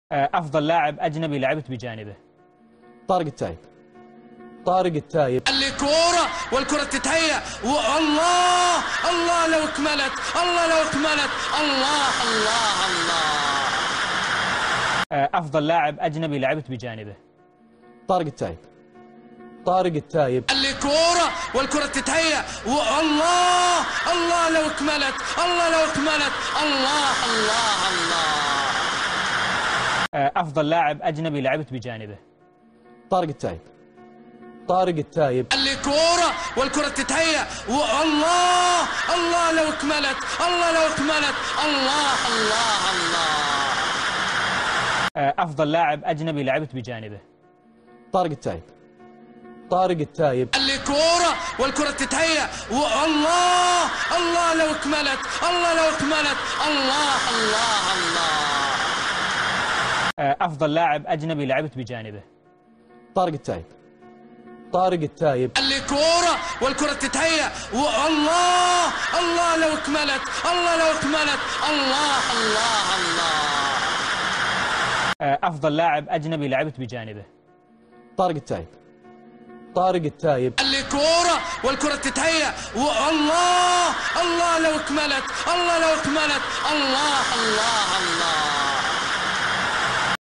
. . تصريح القناص في أفضل لاعب اجنبي لعب بجانبه في غير...
تصريح القناص في أفضل لاعب اجنبي لعب بجانبه 💙💙💙
في غيره !!!
البرنس الليبي 💙💙💙
منشن هلالي يشوف المقطع 😉 💙 .
عمل مع
@x_med11 تابعوه 💙